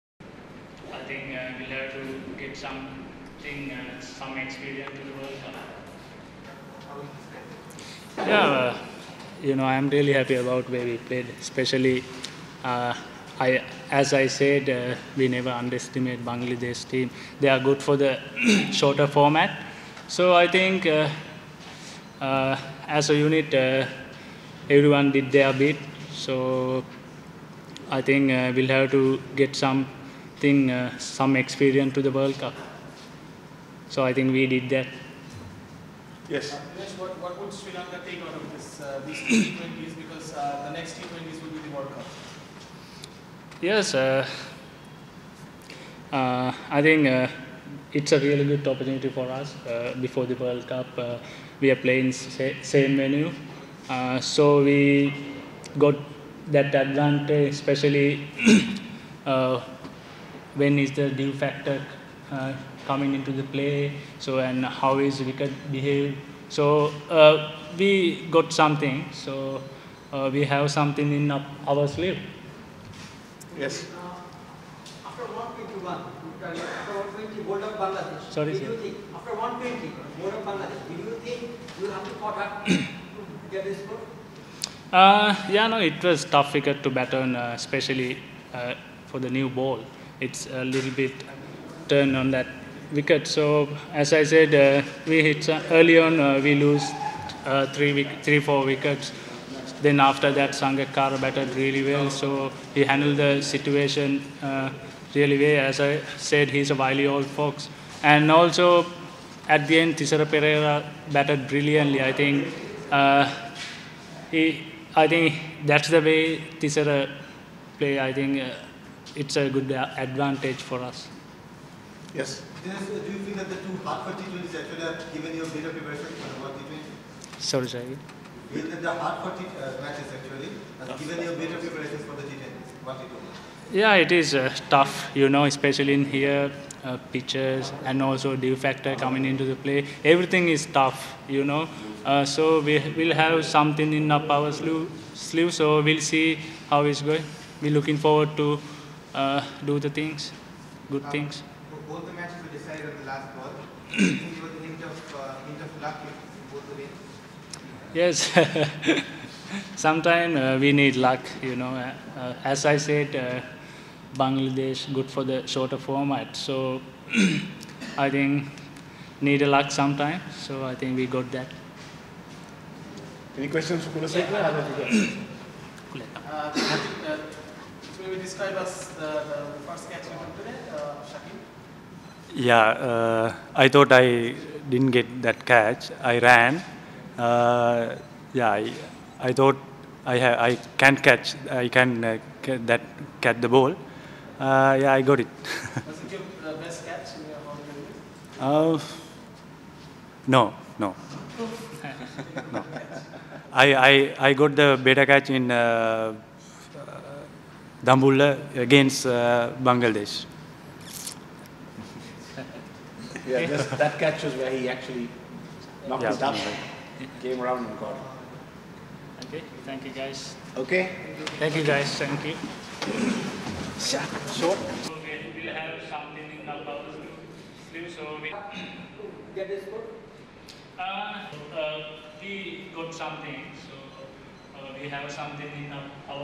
2nd T20I: Post Match Press Conference with Chandimal & Kulasekara - Feb 14th
Sri Lanka secured a dramatic three-wicket win over Bangladesh in the second Twenty20 match in Chittagong on 14th Feb 2014 to win the series 2-0.